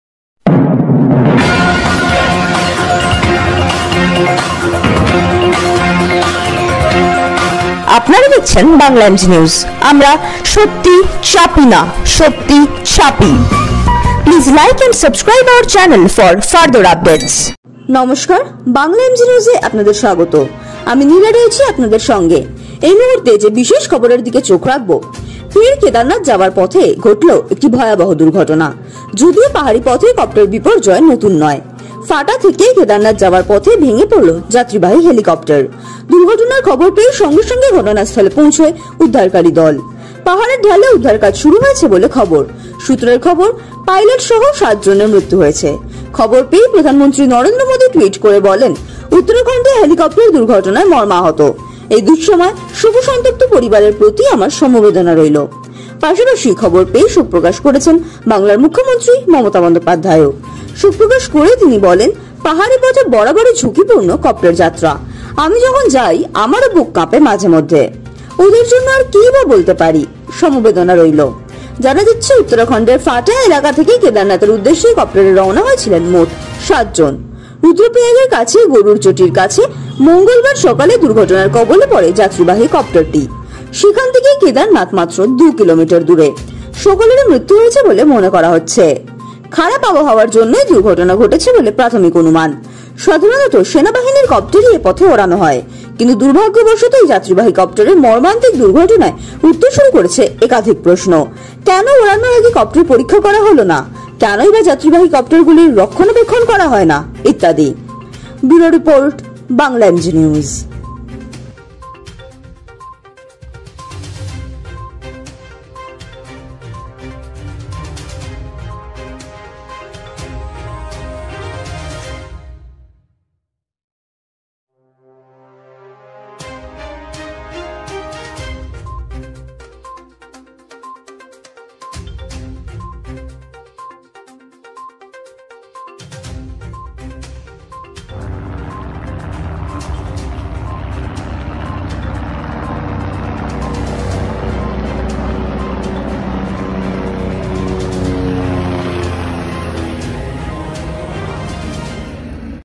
ফের উত্তরাখন্ডে ভয়াবহ কপ্টার দুর্ঘটনা
ফের কেদারনাথ যাওয়ার পথে ঘটলো একটি ভয়াবহ দুর্ঘটনা যদিও পাহাড়ি পথে কপ্টার বিপর্যয় নতুন নয়। ফাটা থেকে কেদারনাথ যাওয়ার পথে ভেঙে পড়ল যাত্রীবাহী হেলিকপ্টার। দুর্ঘটনার খবর পেয়ে সঙ্গে সঙ্গে ঘটনাস্থলে পৌঁছেছে উদ্ধারকারী দল। পাহাড়ের ঢালে উদ্ধার কাজ শুরু হয়েছে বলে খবর। সূত্রের খবর, পাইলট সহ ৭ জনের মৃত্যু হয়েছে। খবর পেয়েই প্রধানমন্ত্রী নরেন্দ্র মোদী টুইট করে বলেন, "উত্তরাখণ্ডে হেলিকপ্টার দুর্ঘটনায় মর্মাহত। এই দুঃসময়ে শোকসন্তপ্ত পরিবারের প্রতি আমার সমবেদনা রইলো।" পাশাপাশি খবর পেয়ে শোকপ্রকাশ করেছেন বাংলার মুখ্যমন্ত্রী মমতা বন্দ্যোপাধ্যায়ও। শোকপ্রকাশ করে তিনি বলেন, "পাহাড়ি পথে বরাবরই ঝুঁকিপূর্ণ কপ্টার যাত্রা। আমি যখন যাই, আমারও বুক কাঁপে মাঝেমধ্যে। ওঁদের জন্য আর কী-ই বা বলতে পারি! সমবেদনা রইল।" জানা যাচ্ছে, উত্তরাখণ্ডের ফাটা এলাকা থেকে কেদারনাথের উদ্দেশে কপ্টারে রওনা হয়েছিলেন মোট ৭ জন।  রুদ্রপ্রয়াগের কাছে গরুড় চটির কাছে মঙ্গলবার সকালে দুর্ঘটনার কবলে পড়ে যাত্রীবাহী কপ্টারটি। সেখান থেকে কেদারনাথ মাত্র ২ কিলোমিটার দূরে। সকলেরই মৃত্যু হয়েছে বলে মনে করা হচ্ছে। খারাপ আবহাওয়ার জন্যই দুর্ঘটনা ঘটেছে বলে প্রাথমিক অনুমান। সাধারণত  সেনাবাহিনীর কপ্টারই এ পথে ওড়ানো হয়। কিন্তু দুর্ভাগ্যবশত এই যাত্রীবাহী কপ্টারের মর্মান্তিক দুর্ঘটনায় উঠতে শুরু করেছে একাধিক প্রশ্ন। কেন ওড়ানোর আগে কপ্টার পরীক্ষা করা হল না? কেনই বা যাত্রীবাহী কপ্টারগুলি রক্ষণাবেক্ষণ করা হয় না? ইত্যাদি।

ব্যুরো রিপোর্ট Bangla MG News